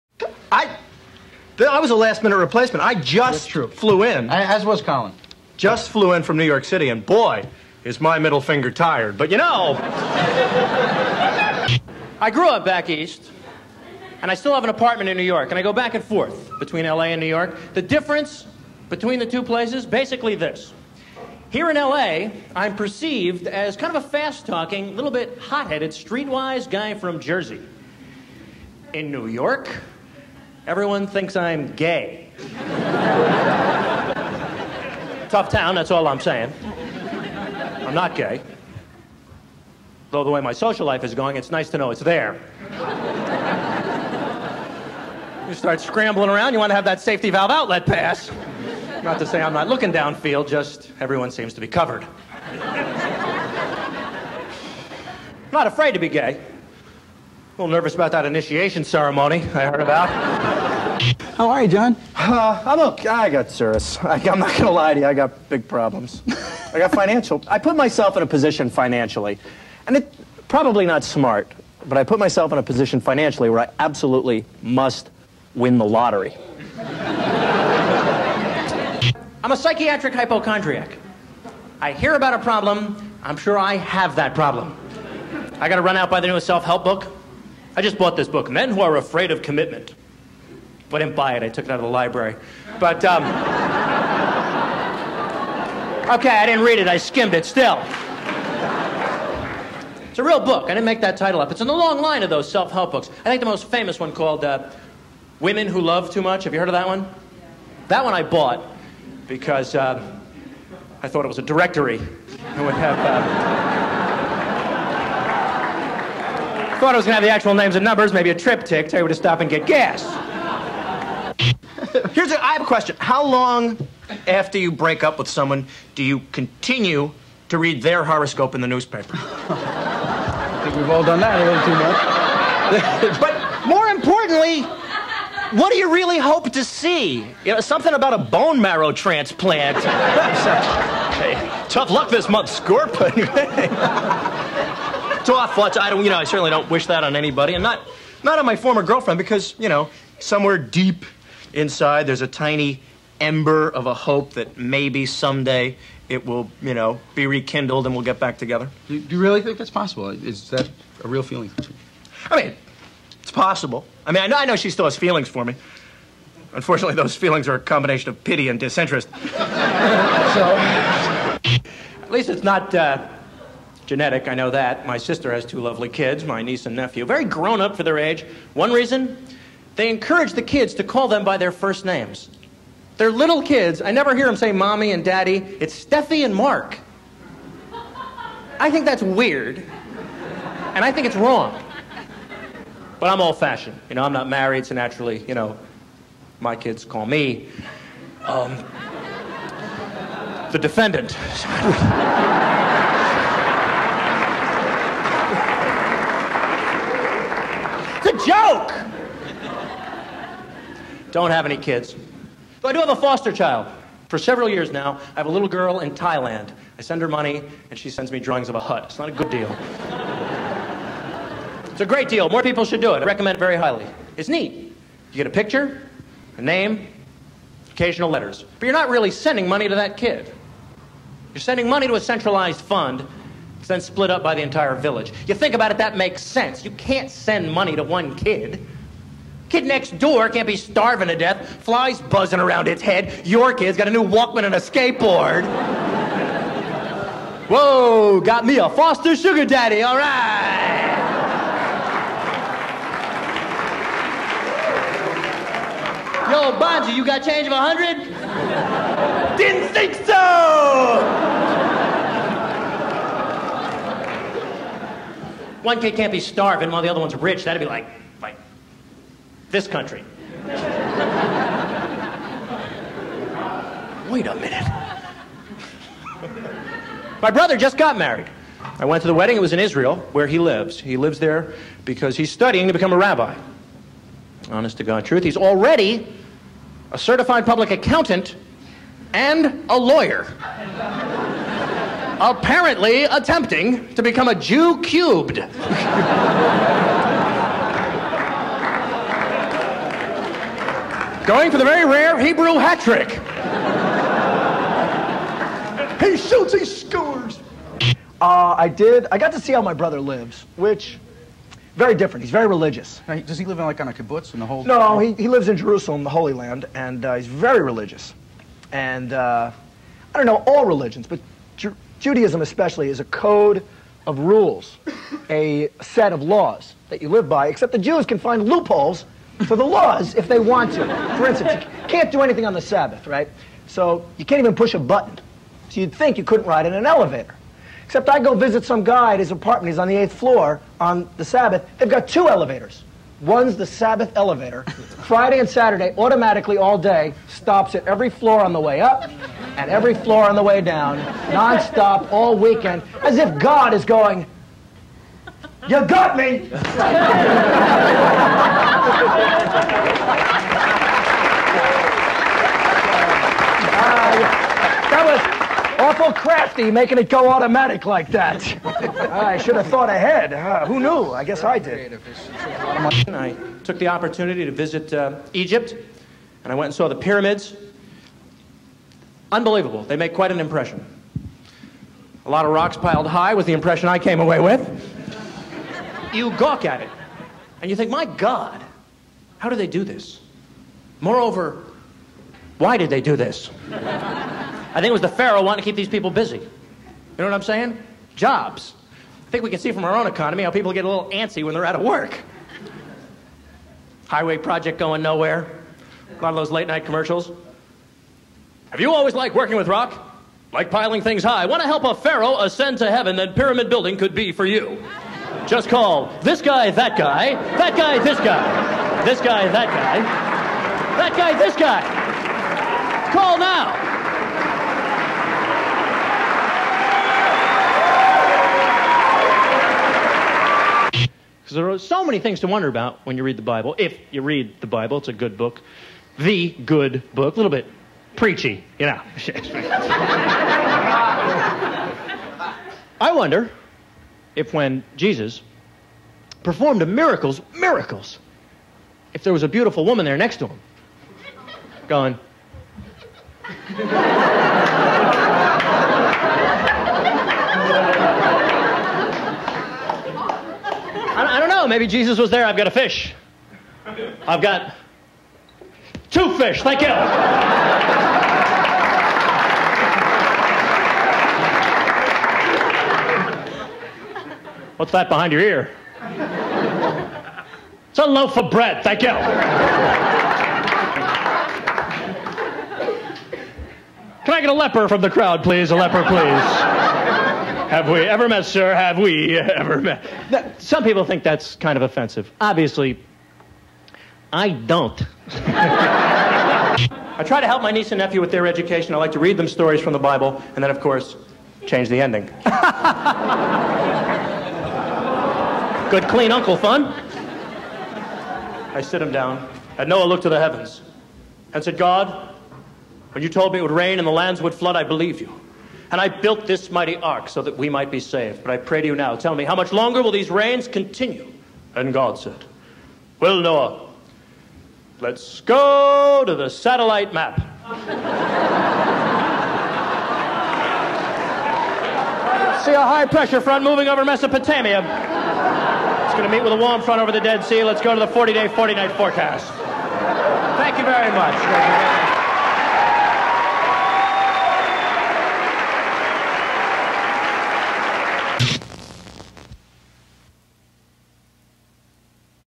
Jon Ross stand-up reel
A compilation of a bunch of different TV appearances.  I had some good material back in the day.  Unfortunately it's tainted by some pretty dubious fashion choices.  Oh '80s, how we loved you.